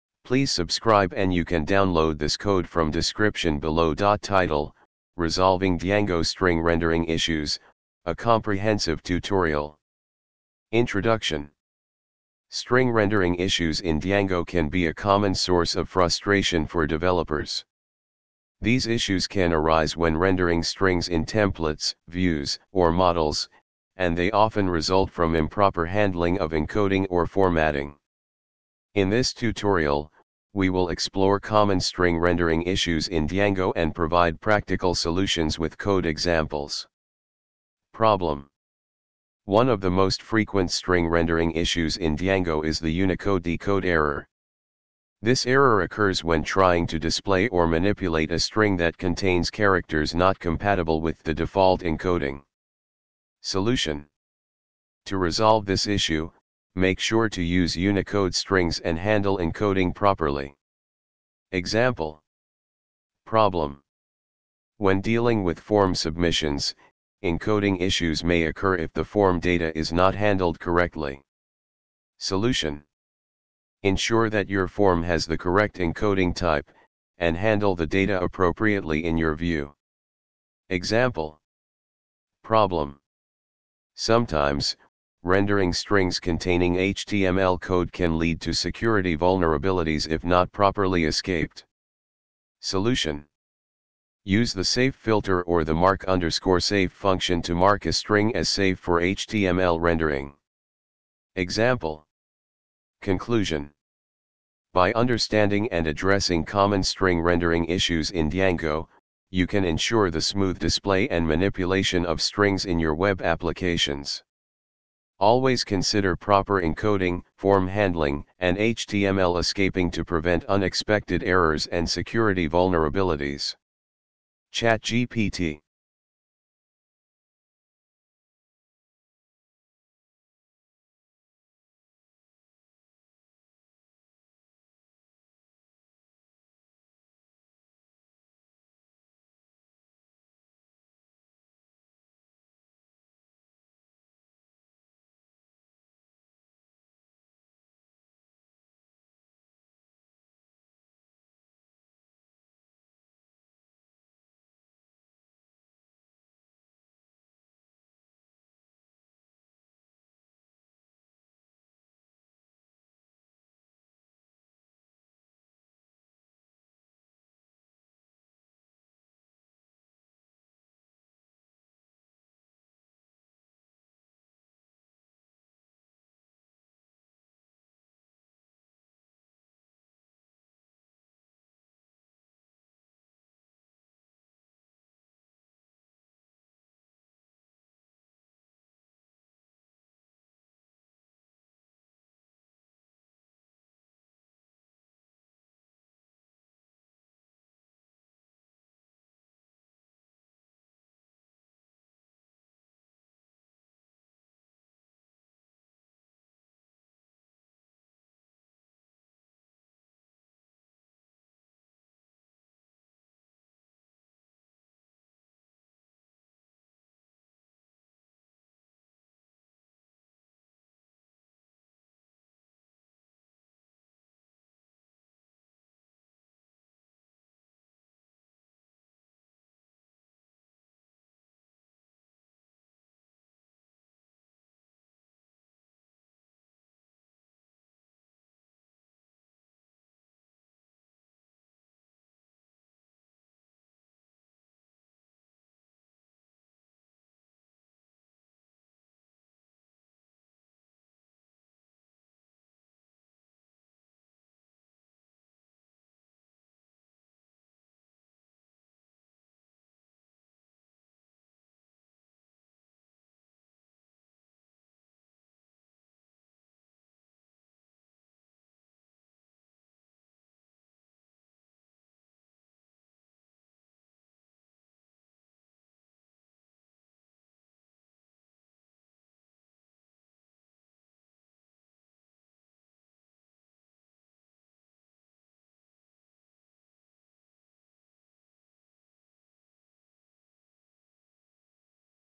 python django string rendering issue
Title: Resolving Django String Rendering Issues: A Comprehensive Tutorial
Introduction:
String rendering issues in Django can be a common source of frustration for developers. These issues can arise when rendering strings in templates, views, or models, and they often result from improper handling of encoding or formatting. In this tutorial, we will explore common string rendering issues in Django and provide practical solutions with code examples.
Problem:
One of the most frequent string rendering issues in Django is the UnicodeDecodeError. This error occurs when trying to display or manipulate a string that contains characters not compatible with the default encoding.
Solution:
To resolve this issue, make sure to use Unicode strings and handle encoding properly.
Example:
Problem:
When dealing with form submissions, encoding issues may occur if the form data is not handled correctly.
Solution:
Ensure that your form has the correct encoding type, and handle the data appropriately in your view.
Example:
Problem:
Sometimes, rendering strings containing HTML code can lead to security vulnerabilities if not properly escaped.
Solution:
Use the safe filter or the mark_safe function to mark a string as safe for HTML rendering.
Example:
Conclusion:
By understanding and addressing common string rendering issues in Django, you can ensure the smooth display and manipulation of strings in your web applications. Always consider proper encoding, form handling, and HTML escaping to prevent unexpected errors and security vulnerabilities.
ChatGPT